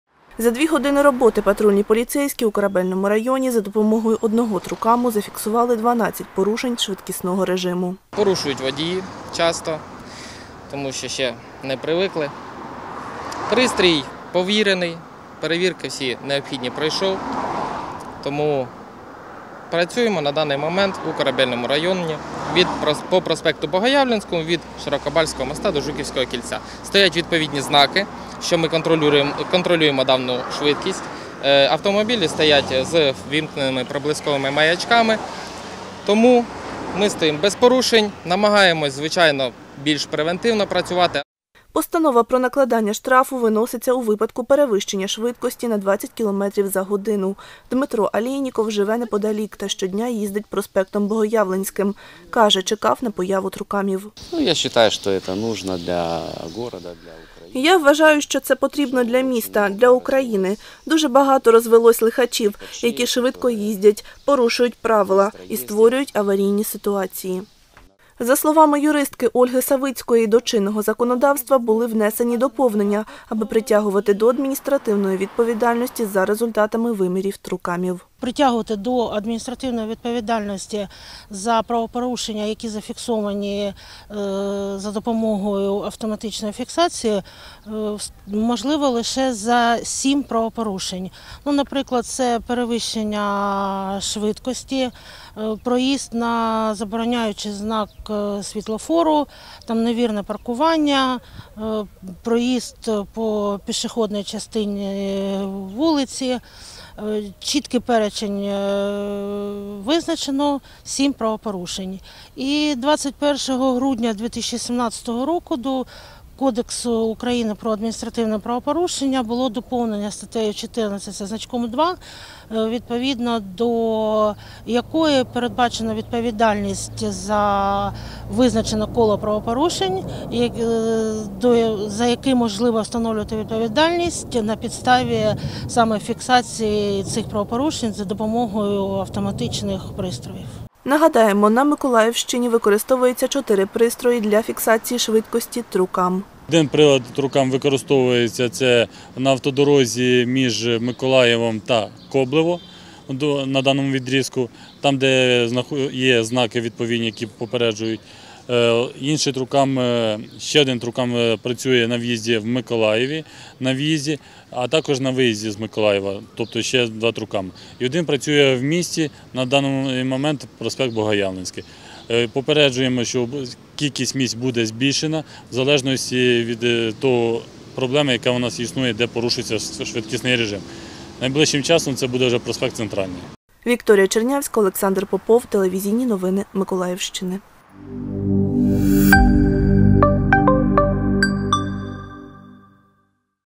За останні два тижні на Миколаївщині зафіксували 228 порушень швидкісного режиму
За останні два тижні з появою двох додаткових приладів TruCam на території Миколаївської області зафіксували 228 порушень швидкісного режиму. Як на місці заміру працюють патрульні поліцейські і чи є результати TruCam  доказом у суді - дізнавались наші журналісти.
Автор:  В. Чернявська
Оператор:  О. Попов